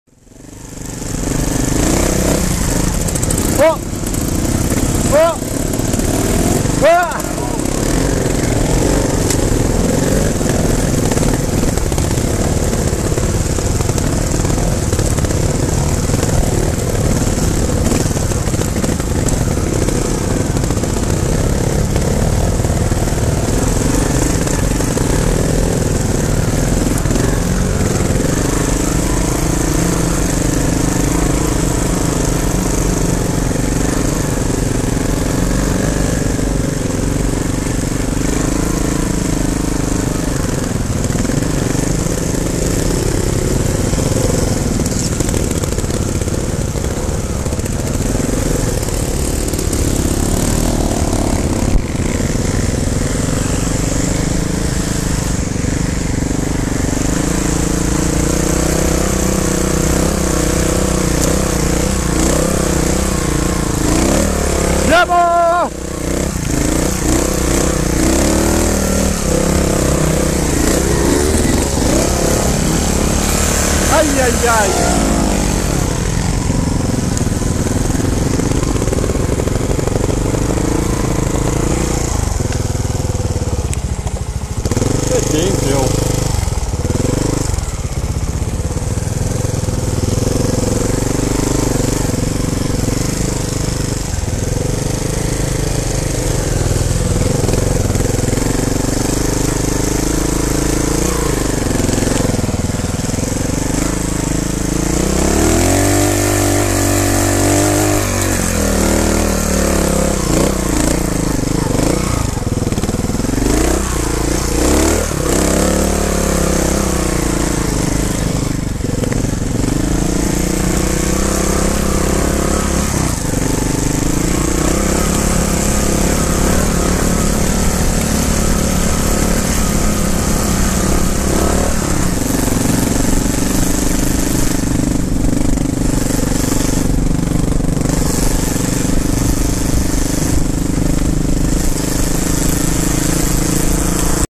Ruzzata pratone Leoni e Lagheé San Lucio
Ben 60 KM di VERO trial con circa 15/18 KG di zaino tra benzina, viveri, ferri, kit emergenza ( che purtroppo ci è servito ) con collegamento radio d'emergenza tra i due gruppi